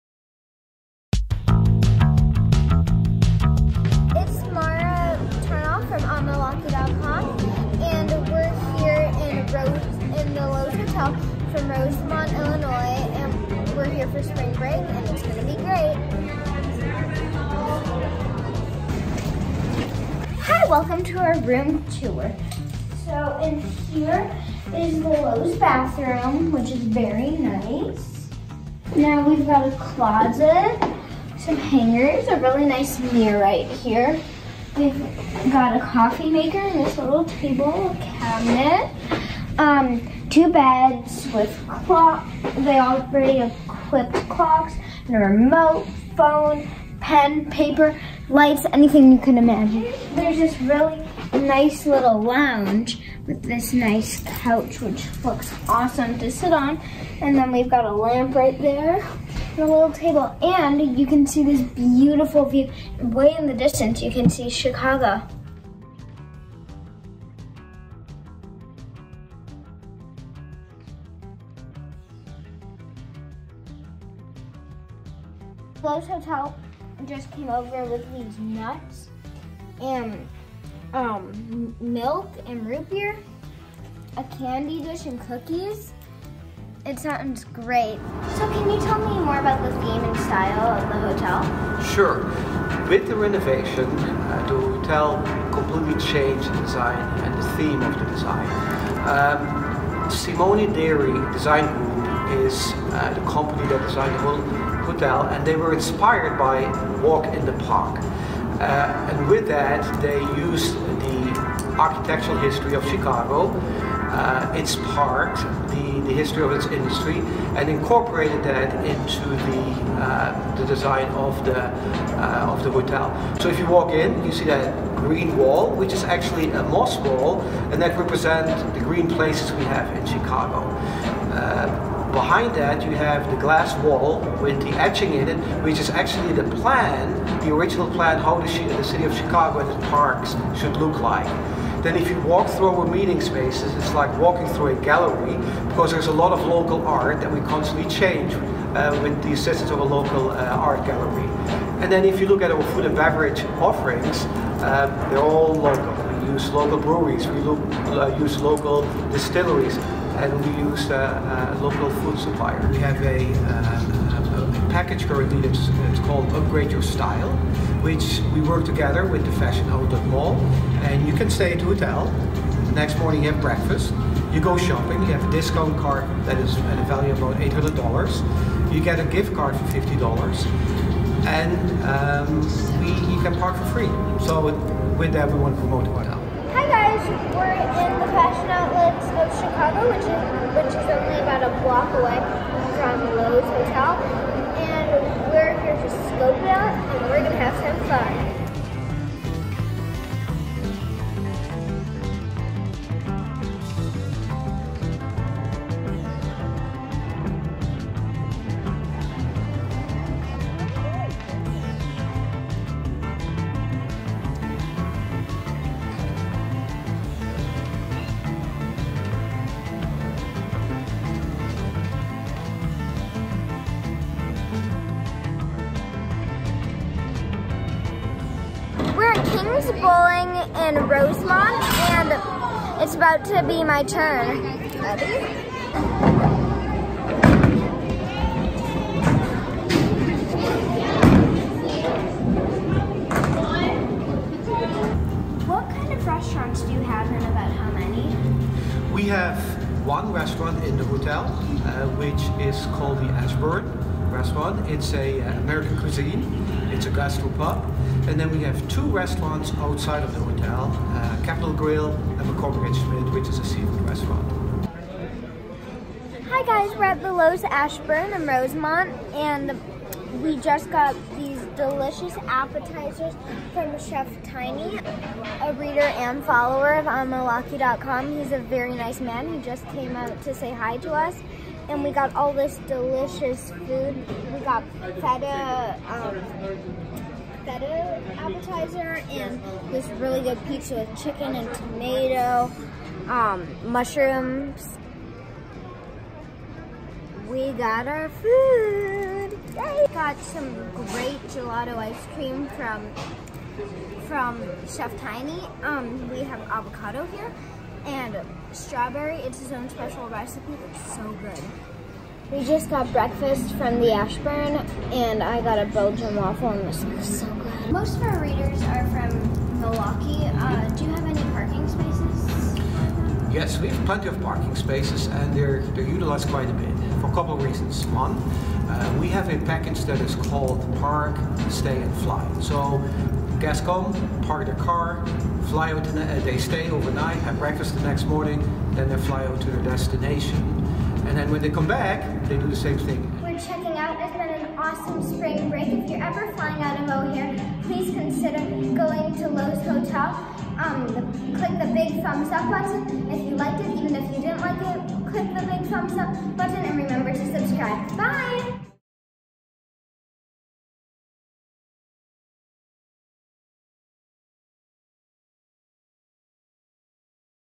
Spring break, Rosemont: Loews Chicago O'Hare Hotel and beyond
Our fourth grade correspondent Mara Tarnoff checks out the newly renovated Loews Chicago O'Hare Hotel for a quick spring break getaway.